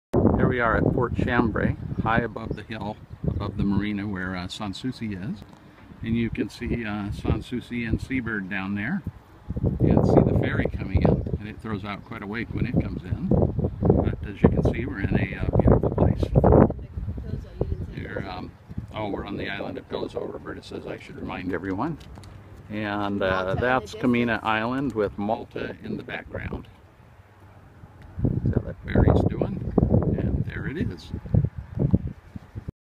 Fort Chambray
Short video taken from a hillside above the marina on Gozo Island, Malta, showing the view, and our boats tied up in the marina.